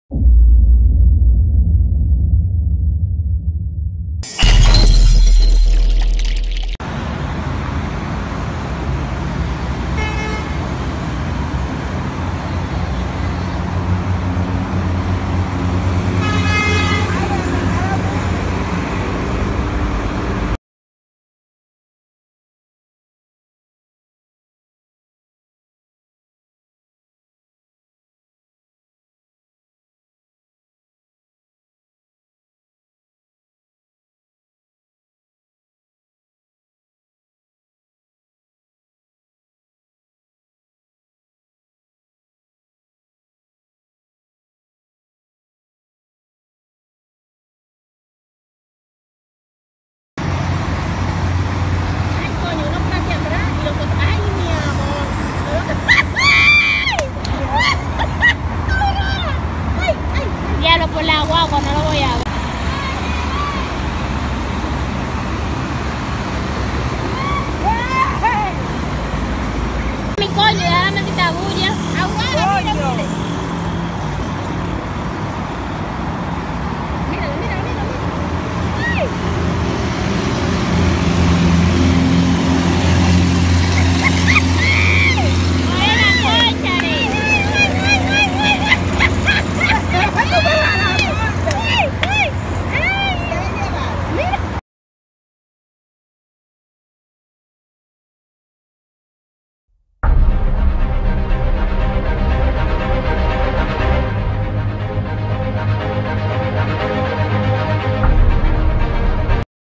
Robo en el kilometro 9 1/2.
Es captado en vídeo los momentos que varios desaprensivos despojan a transeúntes en el kilometro 9  ½ de la Duarte, en donde a pocos metros hay un destacamento policial.